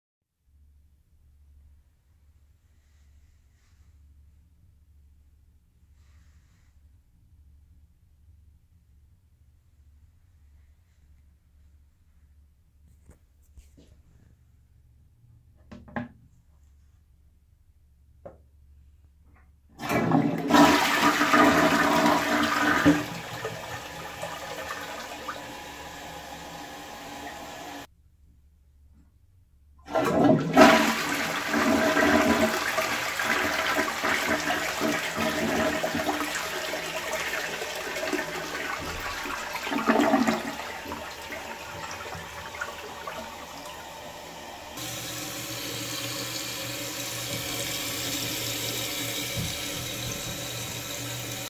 2003 Crane Rexmont
Here's another video from Texas that I forgot to upload. These aren't great but I still like them. This one had a hard time siphoning out. I had to hold the handle down to get it to work properly.